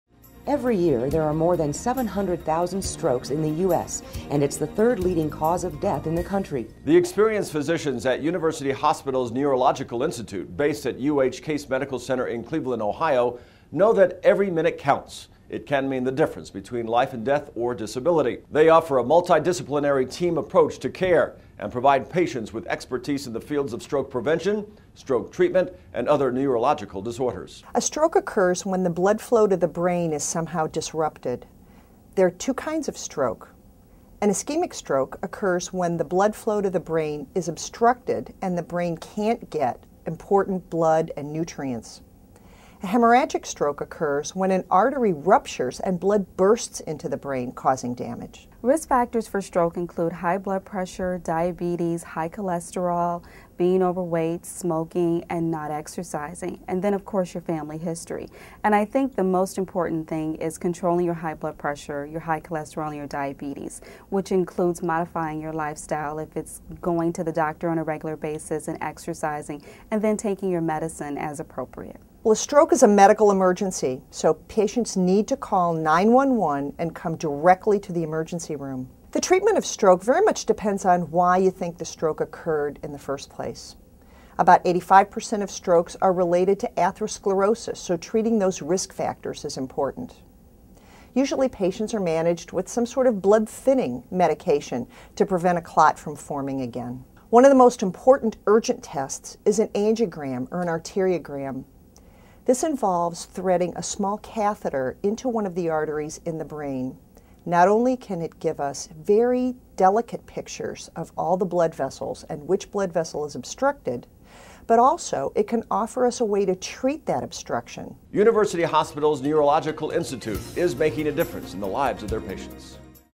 Stroke
Dr. Cathy Sila, UH Neurological Institute, explores the prevention and treatment of ischemic and hemorrhagic strokes.